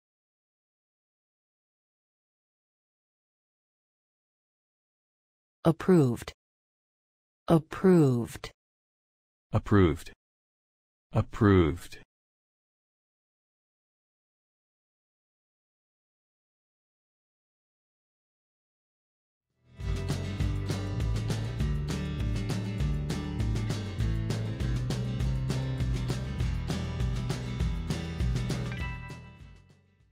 मंजूर की इंग्लिश में क्या है
मंजूर की को अंग्रेजी में सही तरह से कैसे बोलें। ध्यान से सुनो।
मंजूर की का हिंदी से अंग्रेजी में सही अनुवाद क्या है?आप अमेरिकी व्यक्ति की यरह बोलिए।